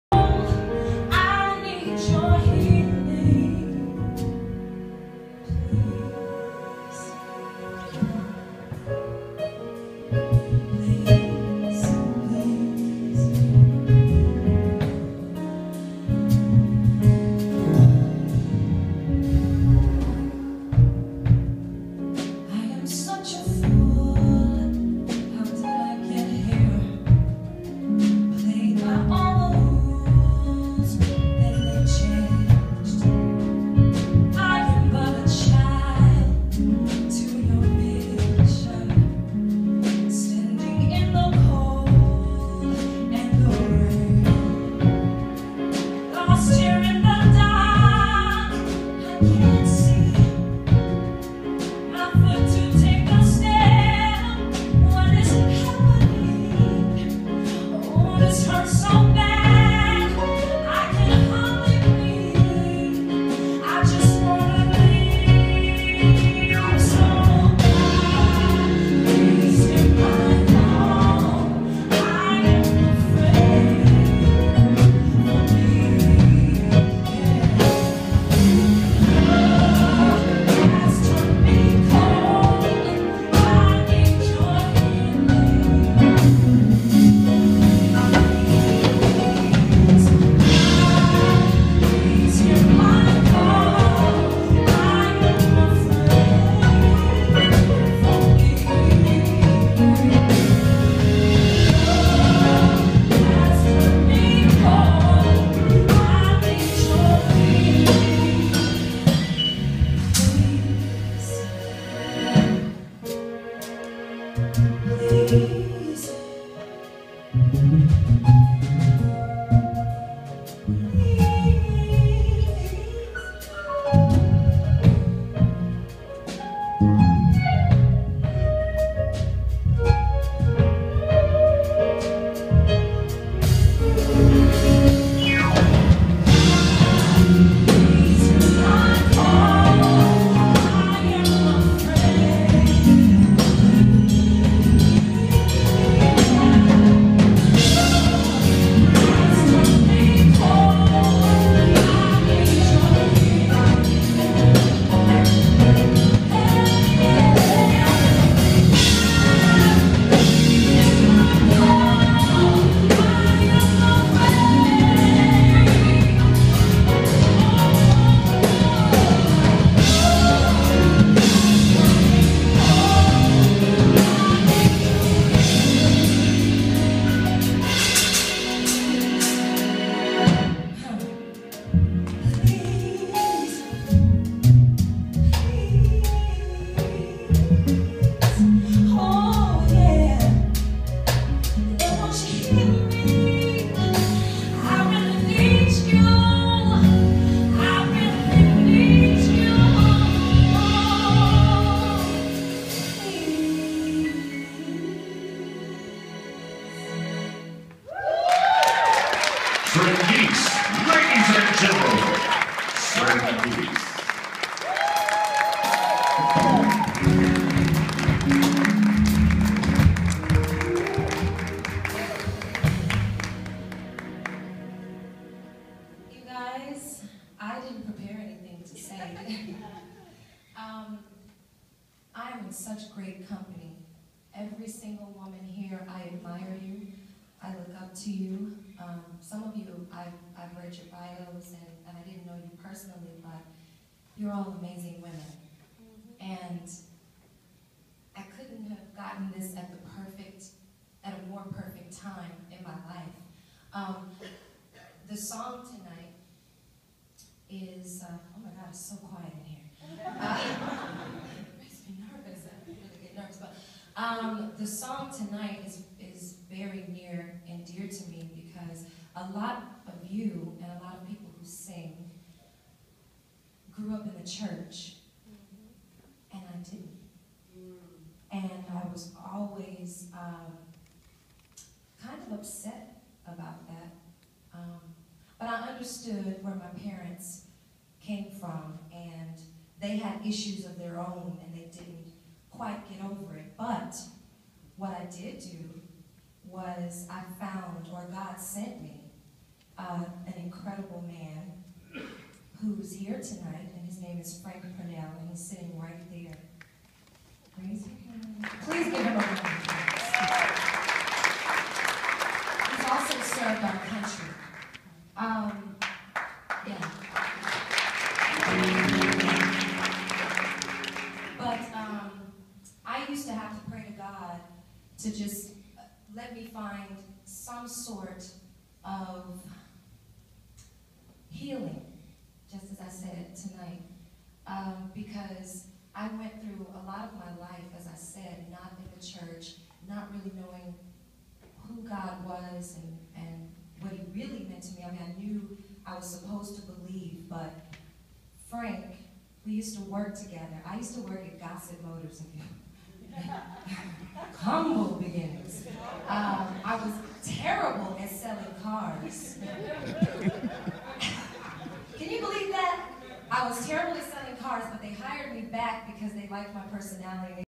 Trenyce Emissaries Awards Performance
Trenyce was honored by her hometown of Memphis TN for her contribution to entertainment on an international level.